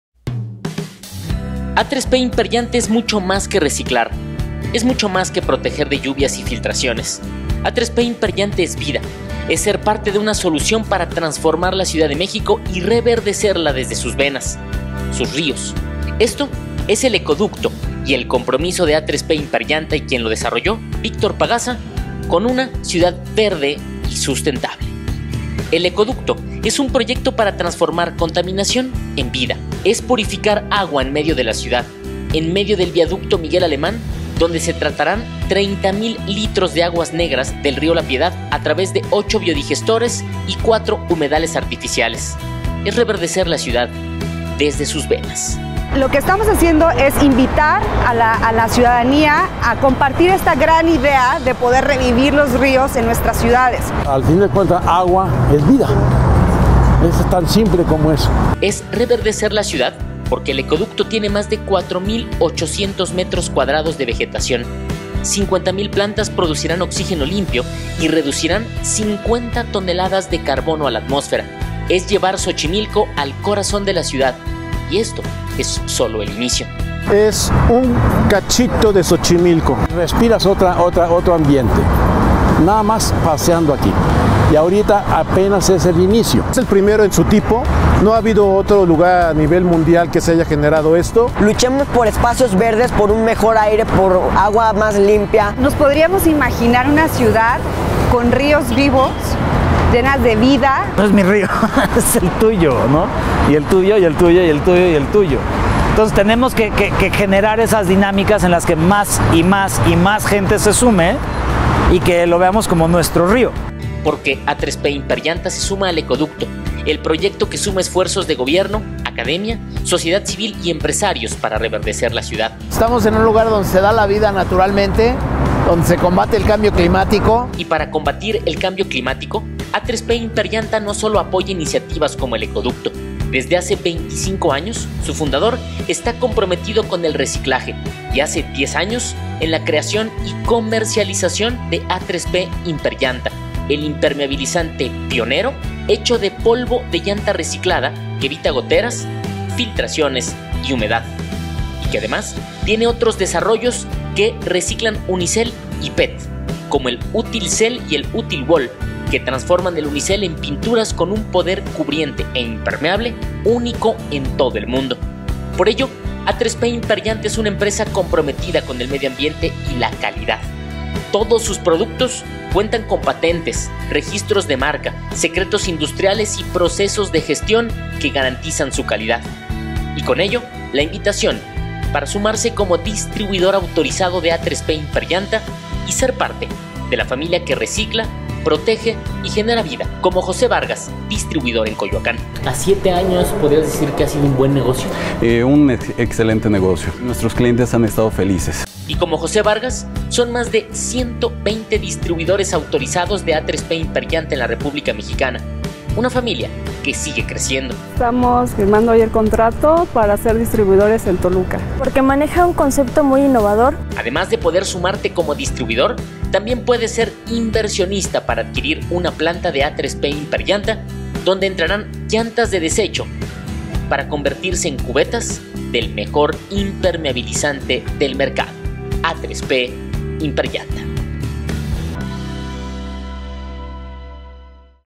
Ecoducto CDMX | Reverdecer la ciudad desde sus venas, desde sus ríos.
¿Qué es el Ecoducto en la cdm y rio Piedad?

¿Cómo la sociedad civil, universidades, académicos y gobierno se unieron en una iniciativa para reverdecer la ciudad?

Conoce y únete a cambiar la ciudad de méxico, hacia la ciudad sustentable.